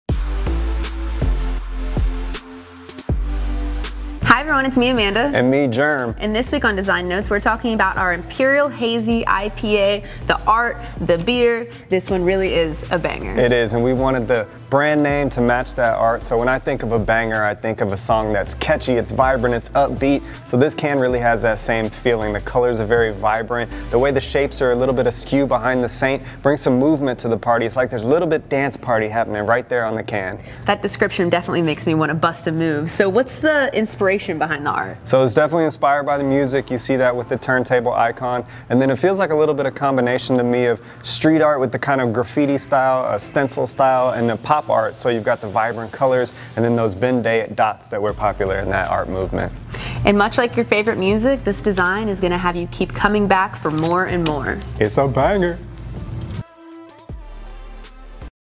Design Notes: Banger IPA
The beer, the art; this one is a Banger!
In this episode of Design Notes, Germ and Amanda are going to share the inspiration behind the Banger IPA design.

When we think of a banger, we think of a song that is high energy, catchy, and gets you moving, and we really wanted a design that matched the name of the beer. The vibrant colors, the icons, the style of art; this one will turn up the volume on your night. It's Hops On Repeat!

Check out this episode of Design Notes to learn more.